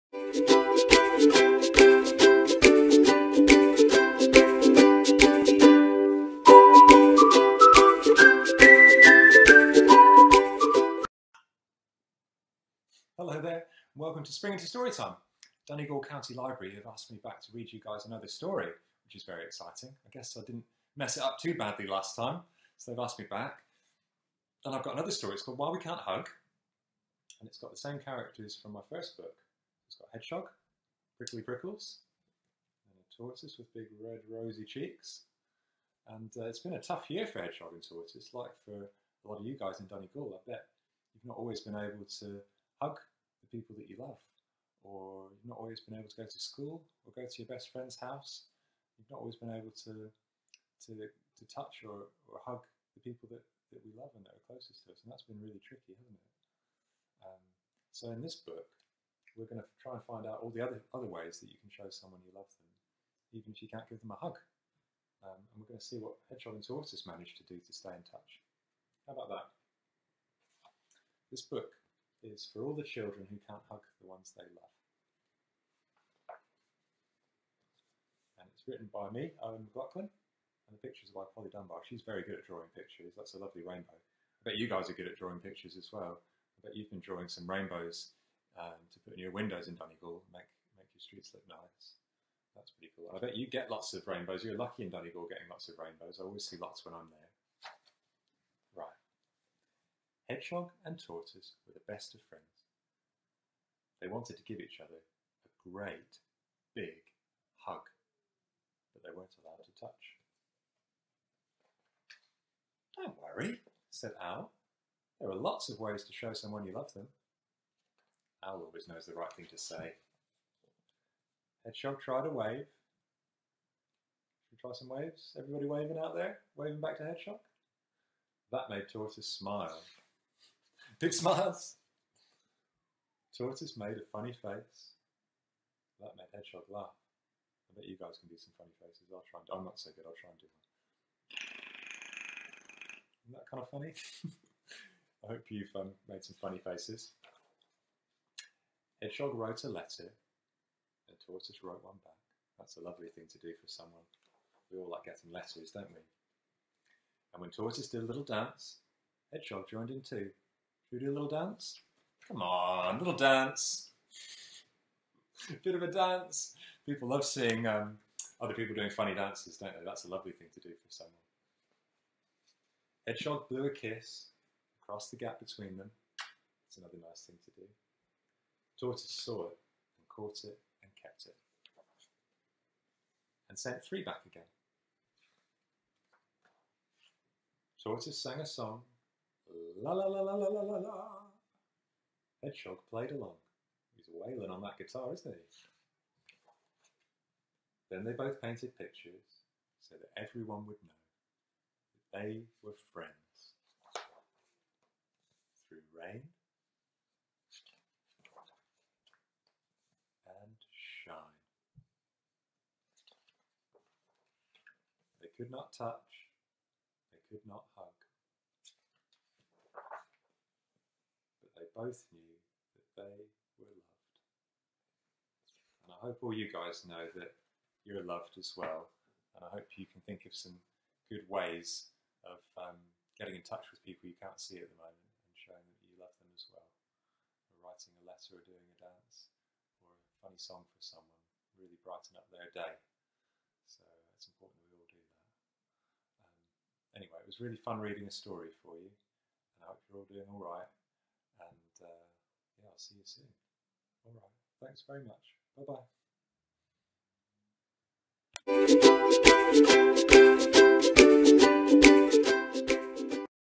Author Eoin McLaughlin When We Can't Hug
Author Eoin McLaughlin reads his wonderful book When We Can't Hug